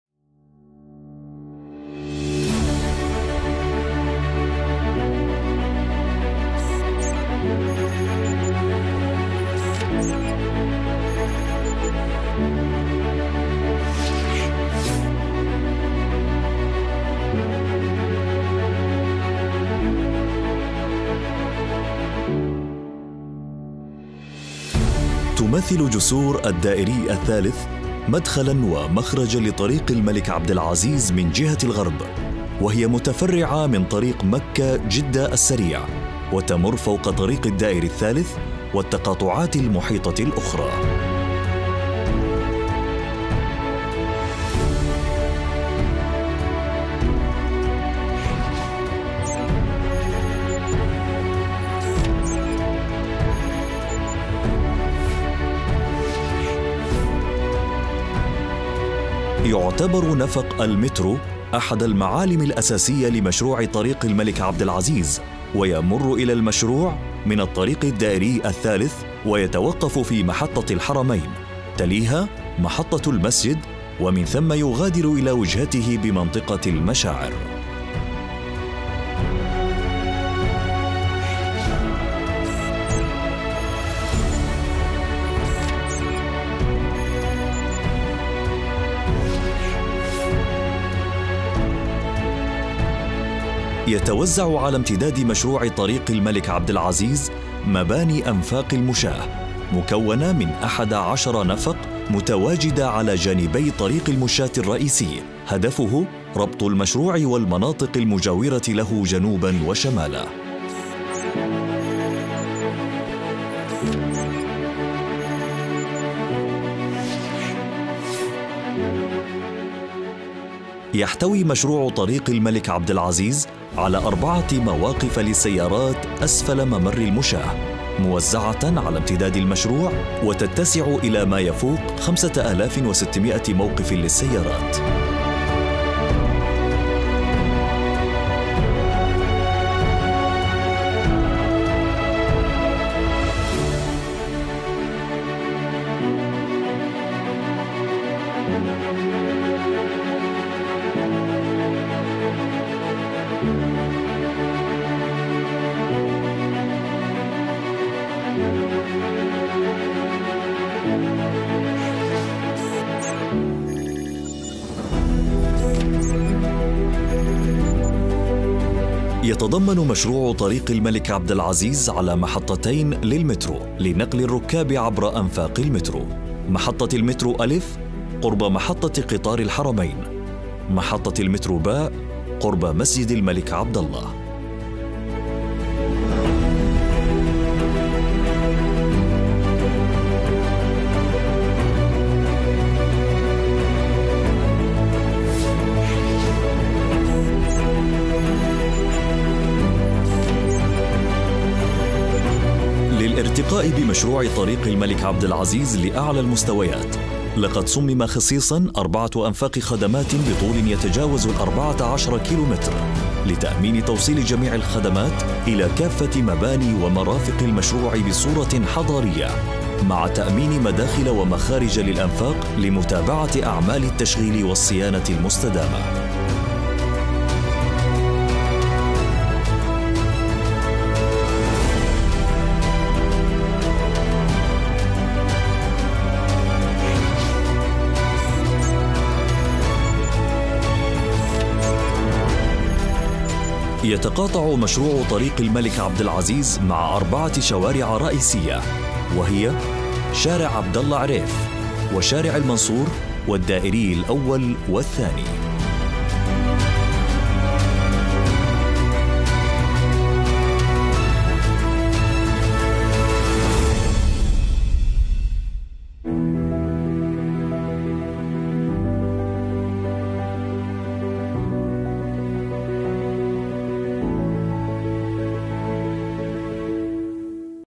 PROJECT REPORT | MASAR | MECCA, KSA | ALTAMASH RAZA KHAN
The MASAR Project is located in the Holy City of Mecca, at the heart of the Islamic culture and its religion. The project comprises over 1.2 million square metres of new development articulated by a central pedestrian boulevard leading to Al Haram Mosque.

Project name: MASAR
Location : Mecca
Equipment Used: ARRI Alexa Classic with Ultra Prime Lenses and Cinema lights
Shot by: Altamash Raza Khan

All Rights are reserved with the respected client